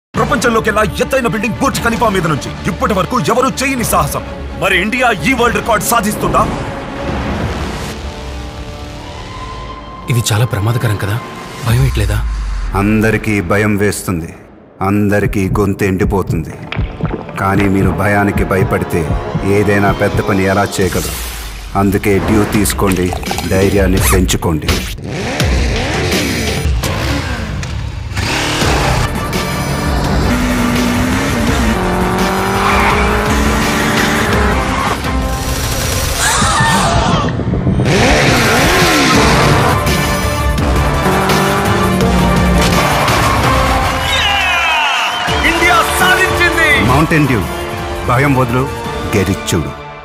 Mountain Dew | New Add | Mahesh Babu Dubai Burj Khalifa
On top of the world! Watch as Dew and I start an epic adventure together!